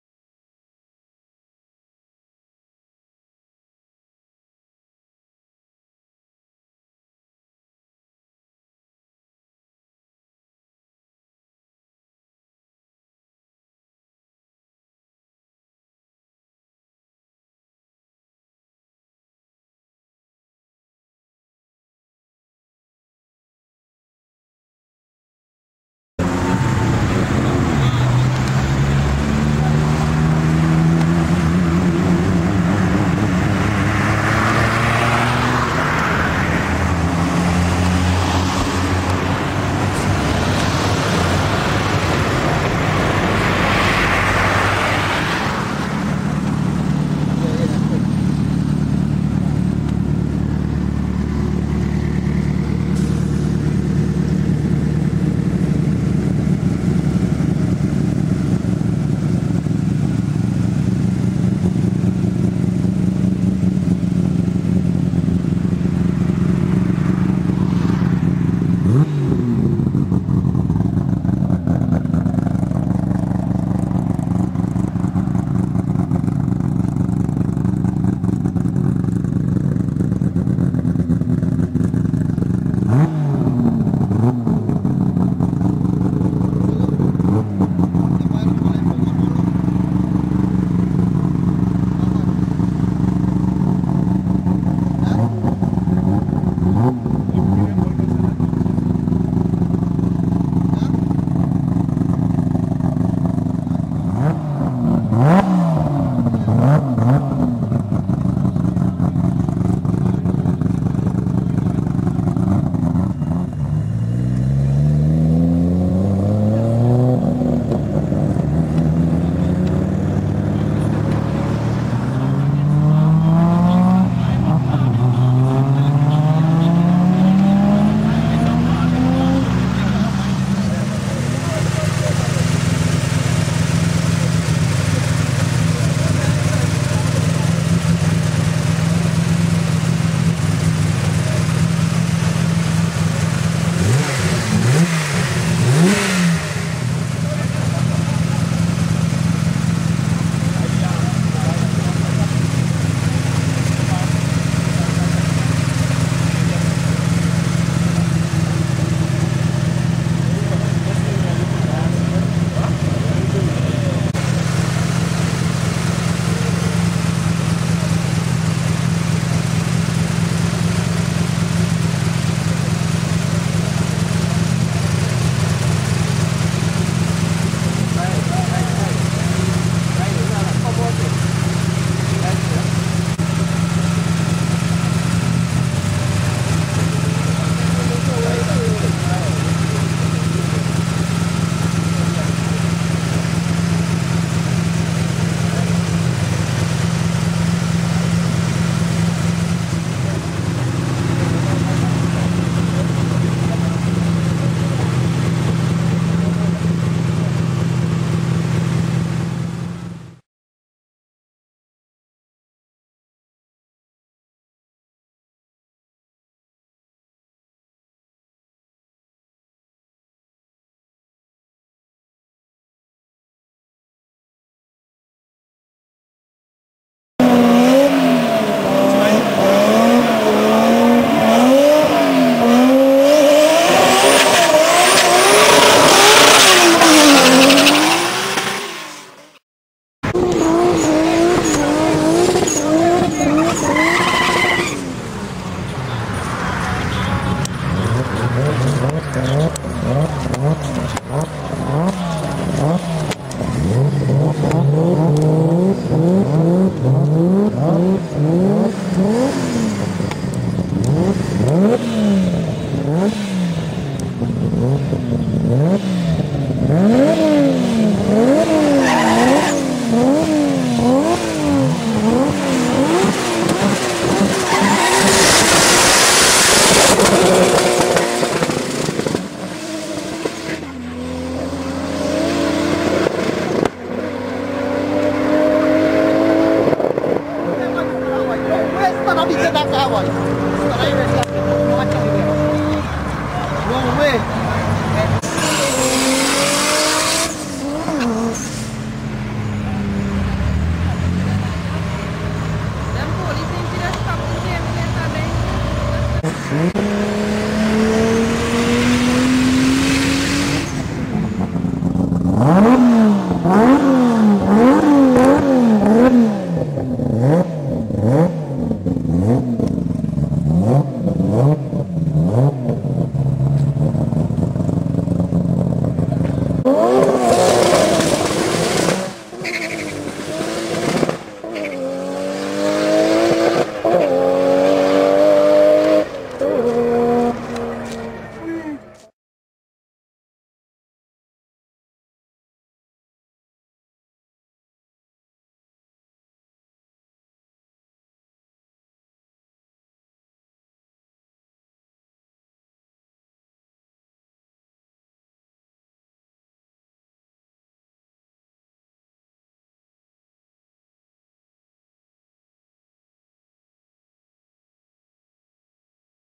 Skyline Vs. Supra
Yancy (Supra)vs Presley (signal auto skyline)
you be the judge of who won!